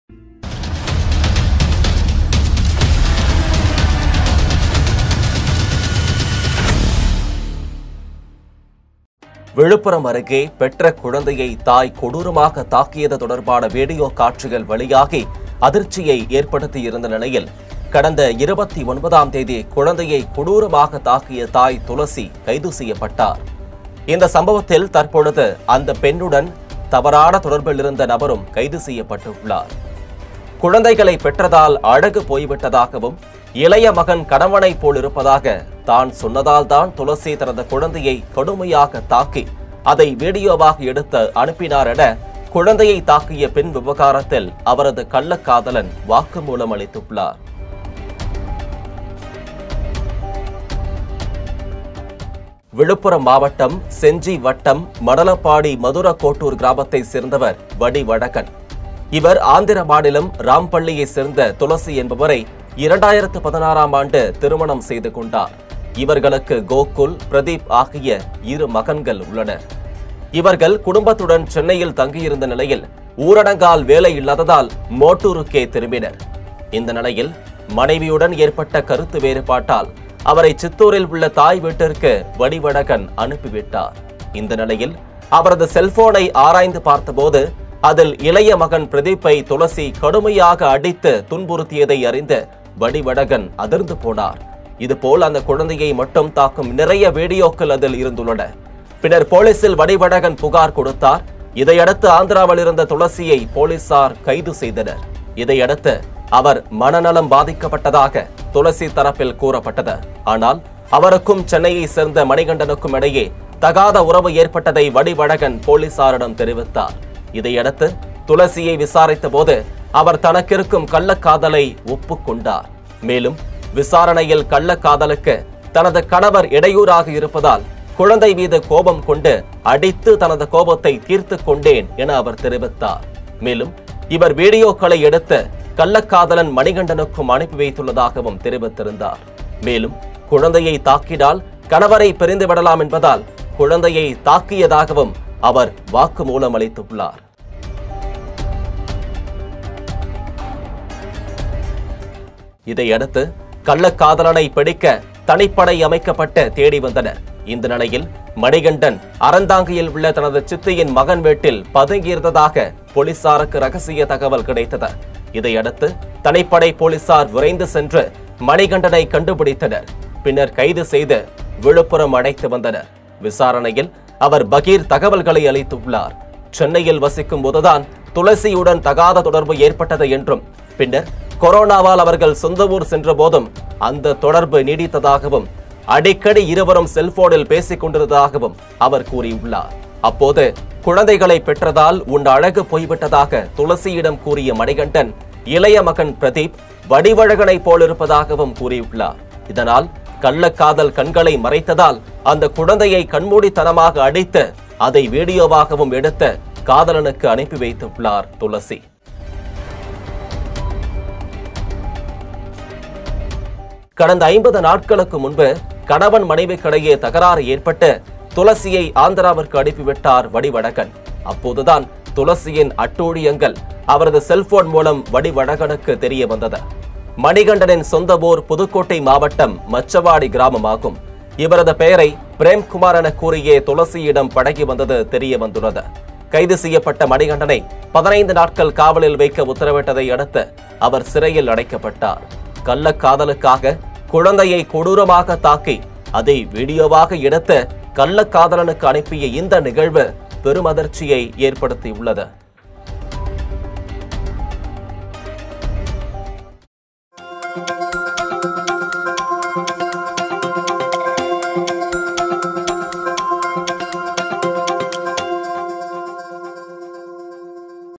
கொடூரத் தாய் துளசி பரபரப்பு வாக்குமூலம் | Villupuram Baby Viral Video | Mother Tulasi | Thulasi Case
கொடூரத் தாய் துளசி பரபரப்பு வாக்குமூலம் : 'கள்ளகாதலன்' வாட்ஸ் அப்பில் பகீர் வீடியோ | Villupuram Baby Viral Video | Mother Tulasi | Thulasi Case